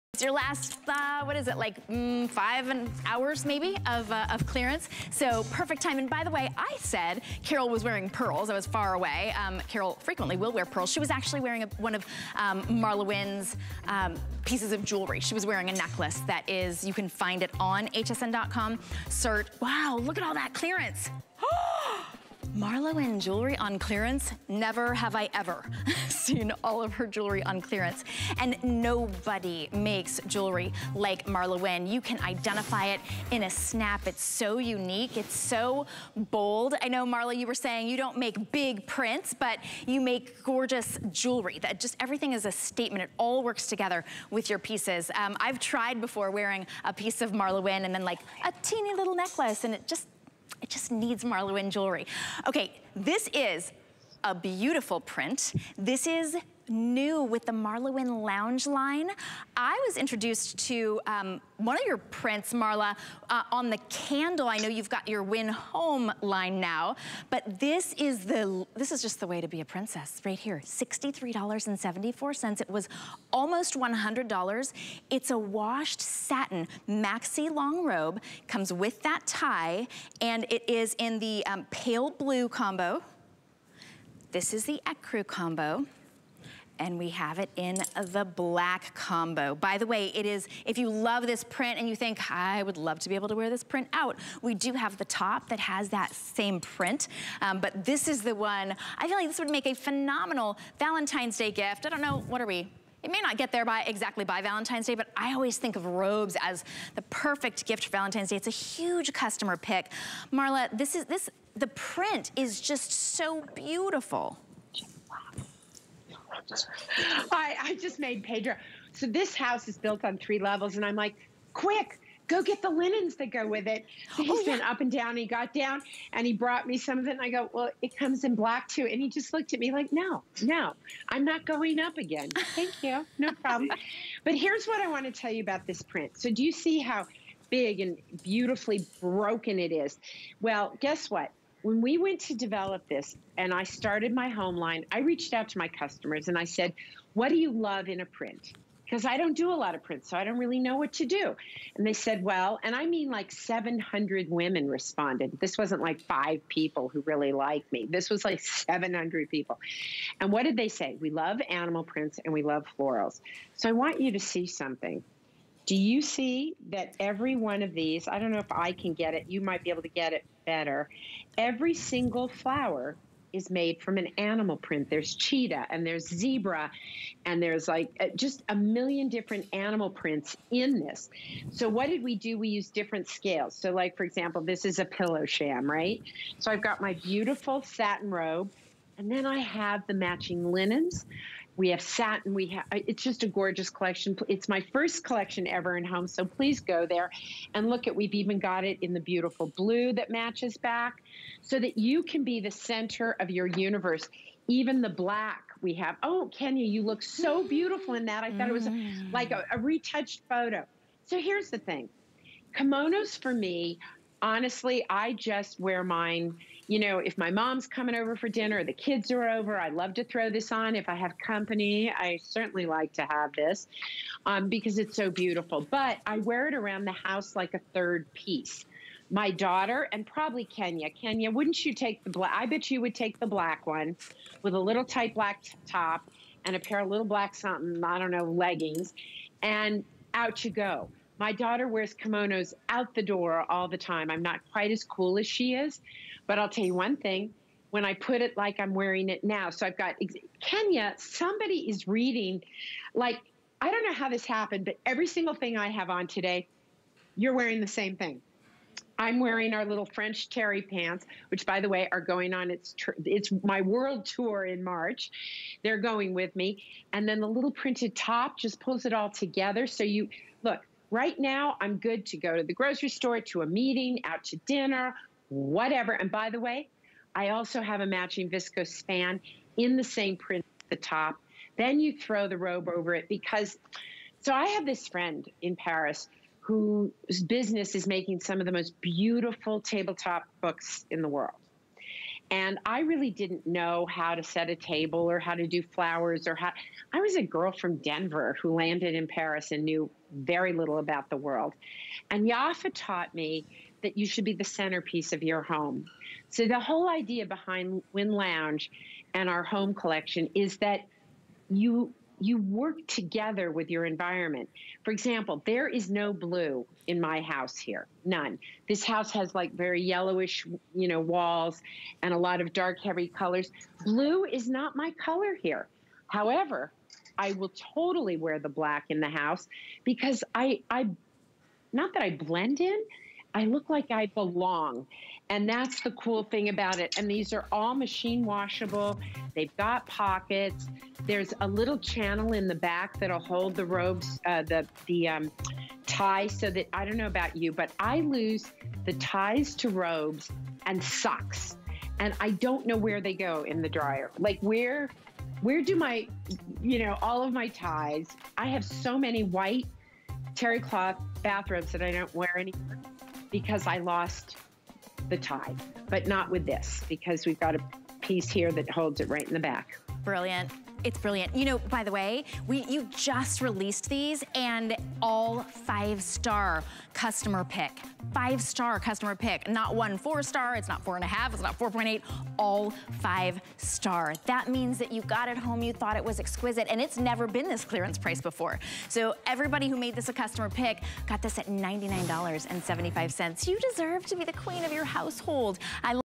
WynneLounge Matte and Shine Washed Satin Maxi Robe
Reimagine your favorite athome looks with this elevated loungewear robe. Washed satin adds a chic, matteandshine effect, while the relaxedfit and wrapstyle, belted tie waist make it easy (not to mention comfortable and stylish) to go from Zoom call to rosallday with (or without) friends in an instant.